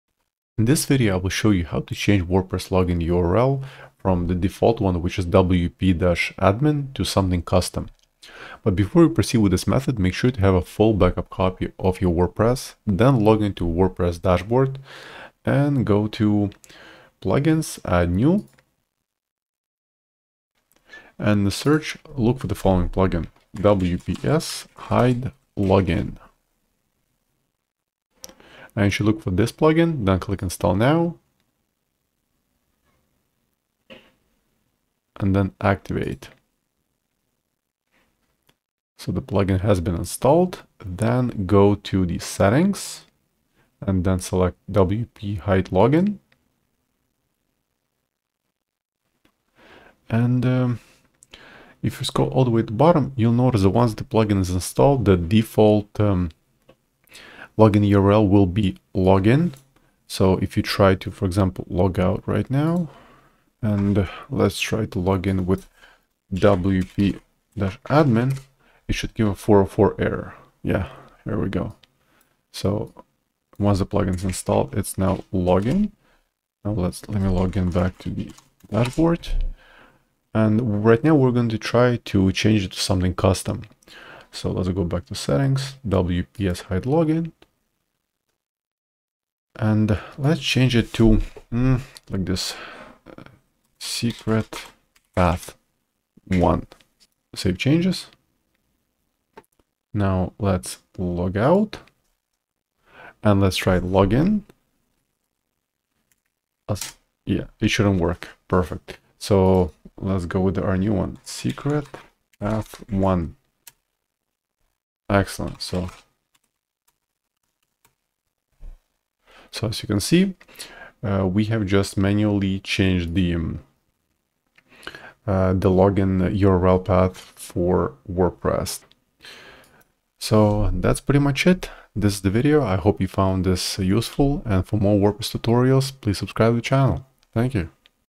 How to Change WordPress Admin Login URL // WPS Hide Login Plugin
In the following WordPress tutorial, Nick demonstrates how to change/hide the default WordPress admin login URL path, such as /wp-admin, /wp-login, /admin, /login.